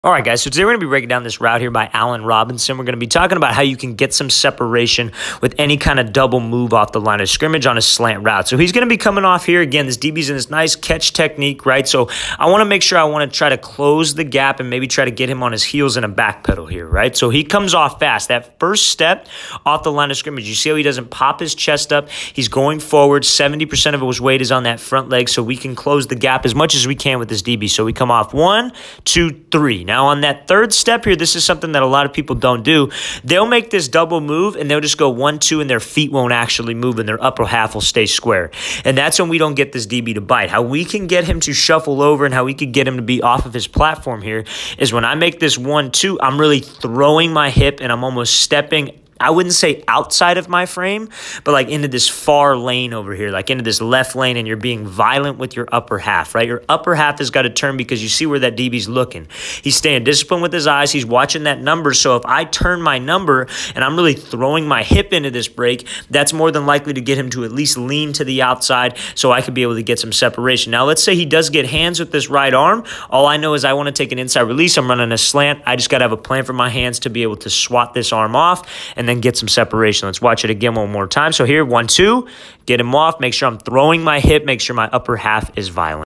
Slant vs. Press Release
In this video I breakdown a release you guys can use when you run a slant route vs press. I hope you enjoy!